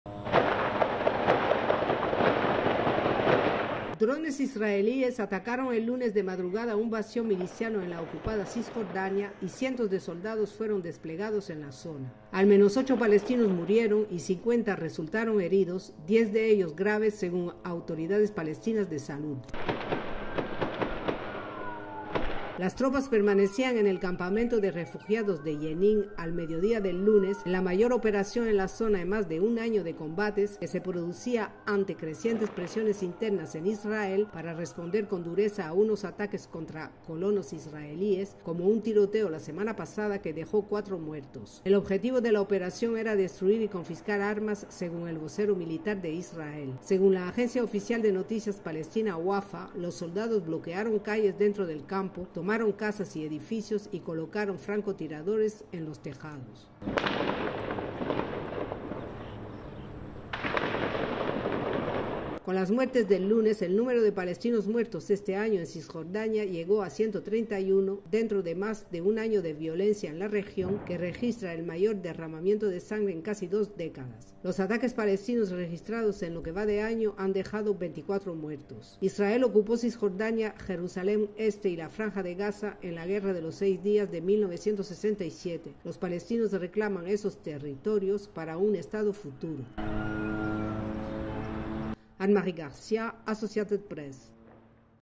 Al menos ocho palestinos muertos en redada de Israel en Cisjordania ocupada
ASSOCIATED PRESS
Yenín, Cisjordania - 3 de julio de 2023

Drones israelíes atacaron el lunes de madrugada un bastión miliciano en la ocupada Cisjordania y cientos de soldados fueron desplegados en la zona.
Al menos ocho palestinos murieron y 50 resultaron heridos, 10 de ellos graves, según autoridades palestinas de salud.
Las tropas permanecían en el campamento de refugiados de Yenín al mediodía del lunes, en la mayor operación en la zona en más de un año de combates que se producía ante crecientes presiones internas en Israel para responder con dureza a unos ataques contra colonos israelíes, como un tiroteo la semana pasada que dejó cuatro muertos.

El objetivo de la operación era destruir y confiscar armas, según el vocero militar de Israel.

Según la agencia oficial de noticias palestina Wafa, los soldados bloquearon calles dentro del campo, tomaron casas y edificios y colocaron francotiradores en los tejados.
Con las muertes del lunes, el número de palestinos muertos este año en Cisjordania llegó a 131, dentro de más de un año de violencia en la región, que registra el mayor derramamiento de sangre en casi dos décadas.
Los ataques palestinos registrados en lo que va de año han dejado 24 muertos.
Israel ocupó Cisjordania, Jerusalén Este y la Franja de Gaza en la Guerra de los Seis Días de 1967. Los palestinos reclaman esos territorios para un estado futuro.